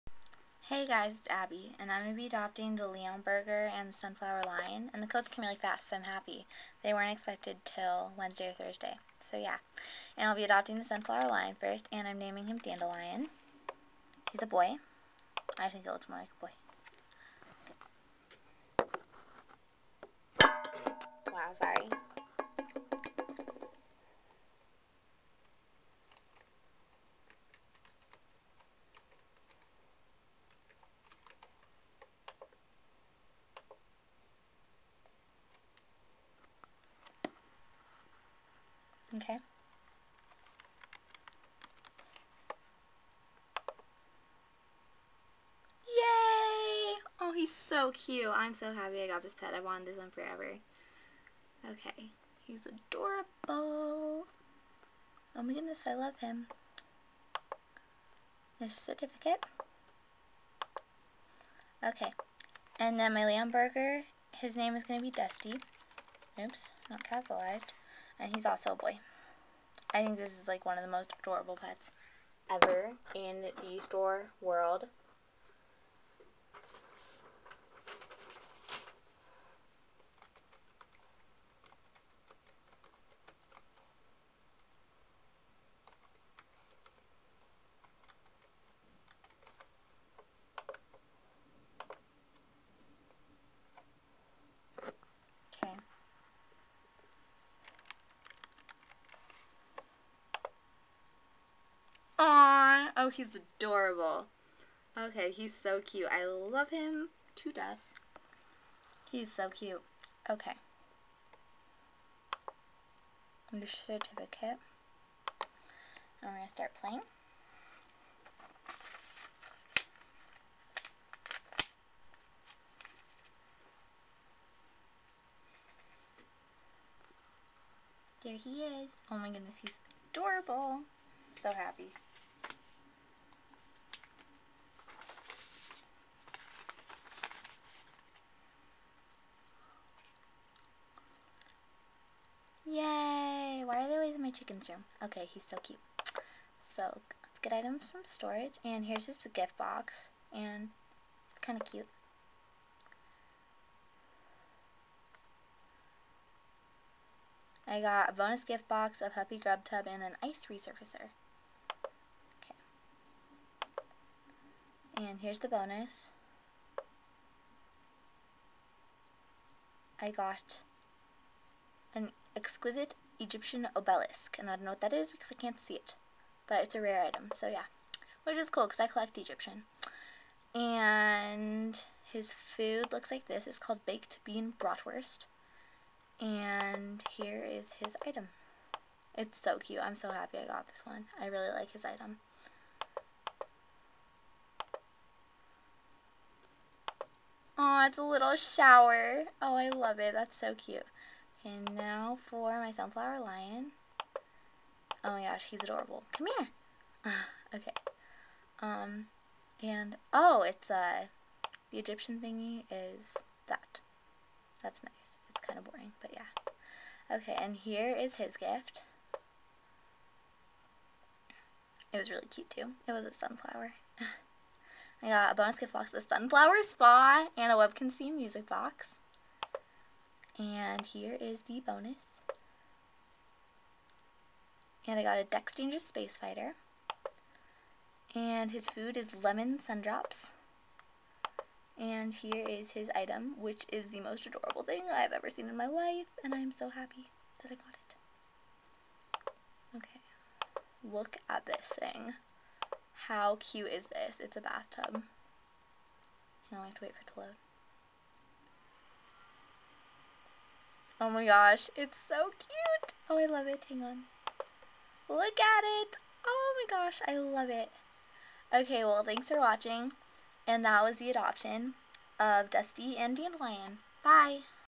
Adopting Dandelion and Dusty!
This is the adoption of my sunflower lion and leonberger!